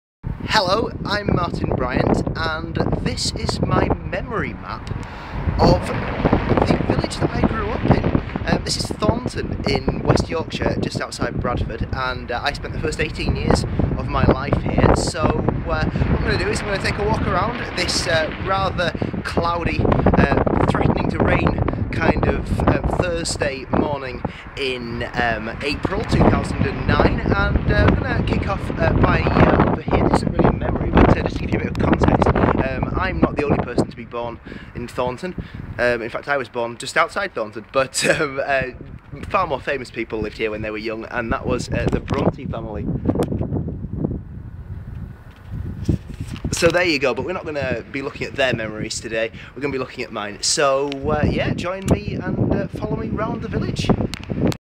Thornton Memory Map part 1
Memories of Thornton linked to a Google Map. Part One - introduction.